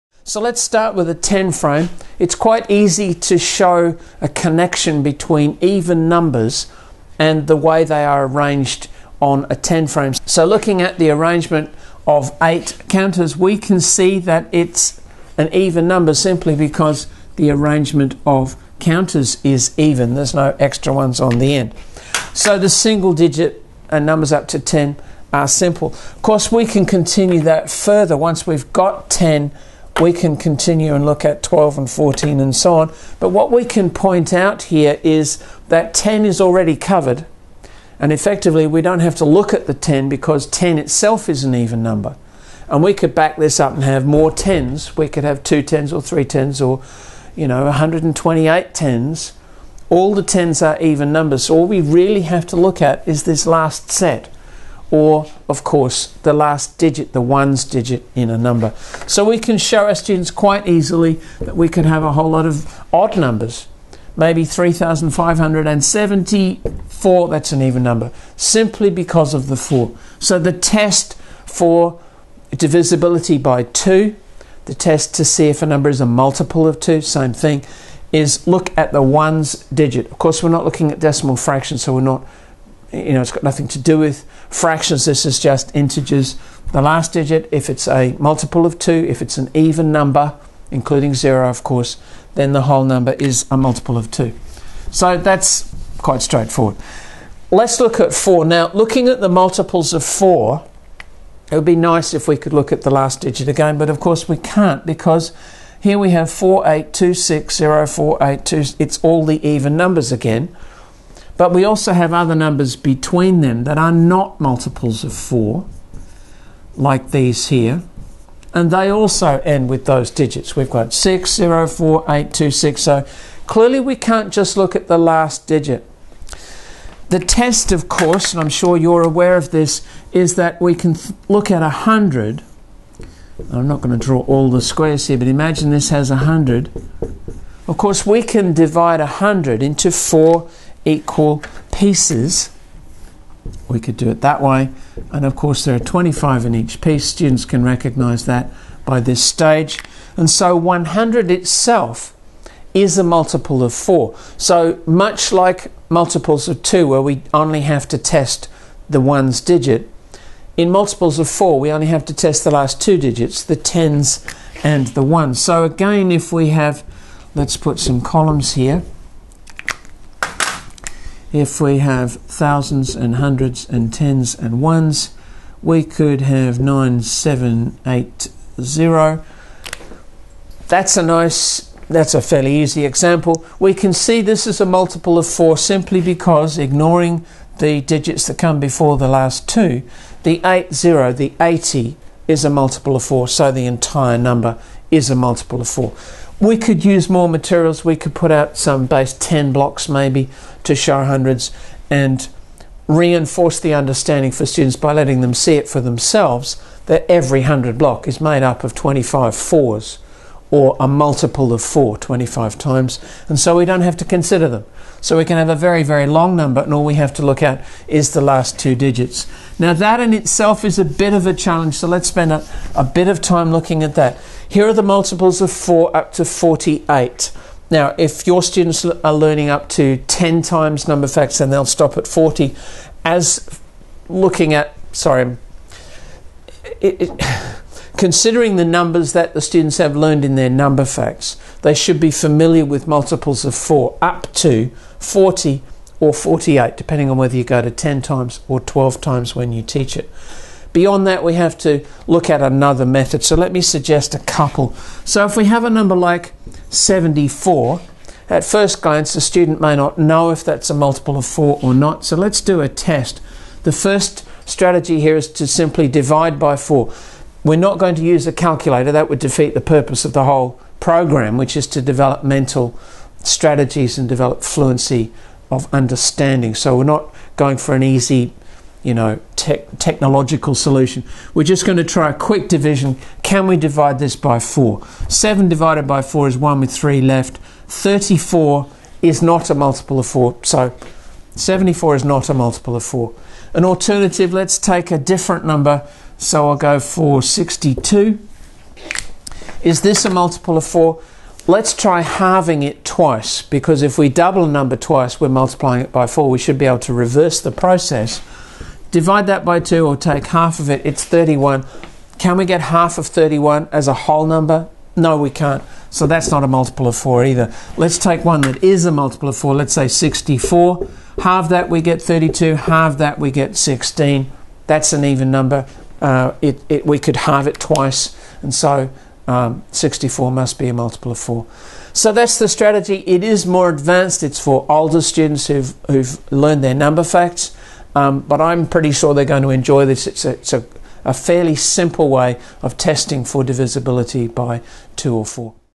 Teach Recognition of Multiples of 2 or 4
The rules of divisibility are quite straight forward, once you know what they are.

Testing if a number is a multiple of 2 involves checking only the final digit. To test for divisibility by 4, check the final two digits.

How would you check if a number was divisible by 8?